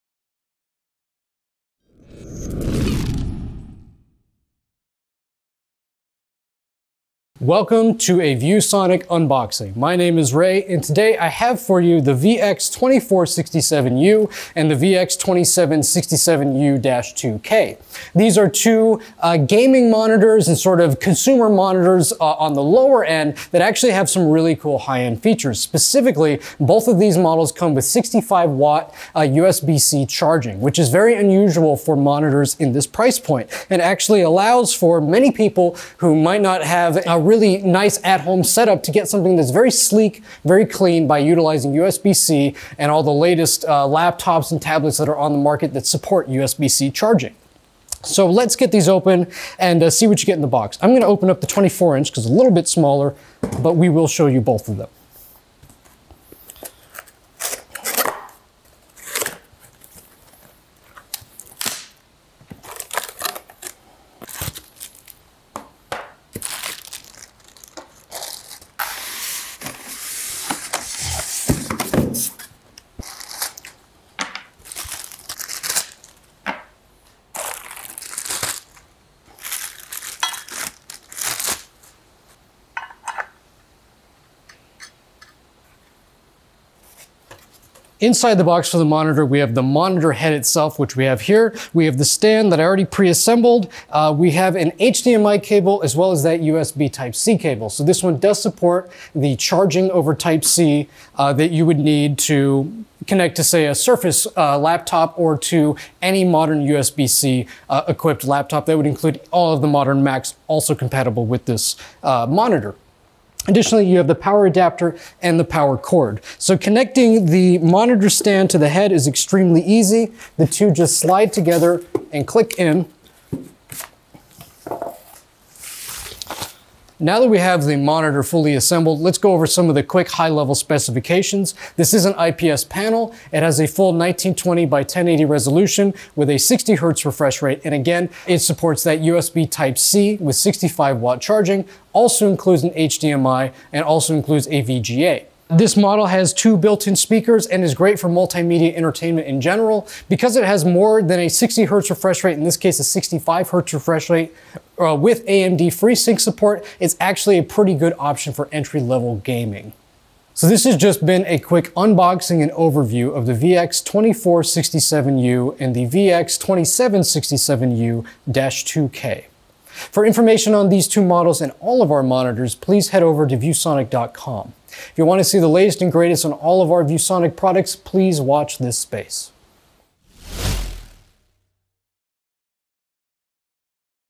Unboxing the VX2467U 24" 1080p IPS Monitor with 65W USB Type-C and HDMI
The ViewSonic VX2467U is a 24" Full HD monitor that combines style with optimal versatility and functionality. Whether for streaming, editing or gaming, these sleek and slim displays are engineered with the everyday user in mind.